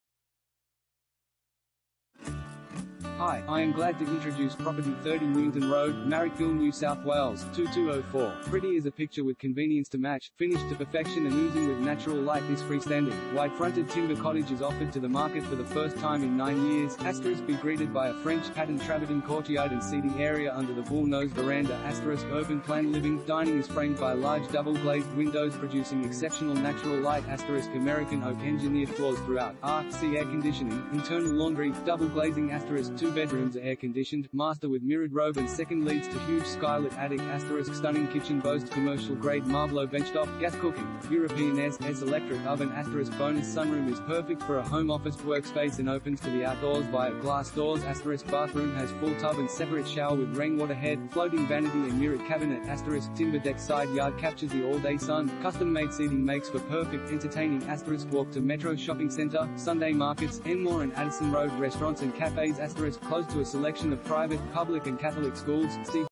30 Newington Road, Marrickville NSW 2204 | AuPropertyReport.Com
30 Newington Road, Marrickville NSW 2204
Property Type: house
Bedrooms: 2
Bathrooms: 2
Car Space: 0
Pretty as a picture with convenience to match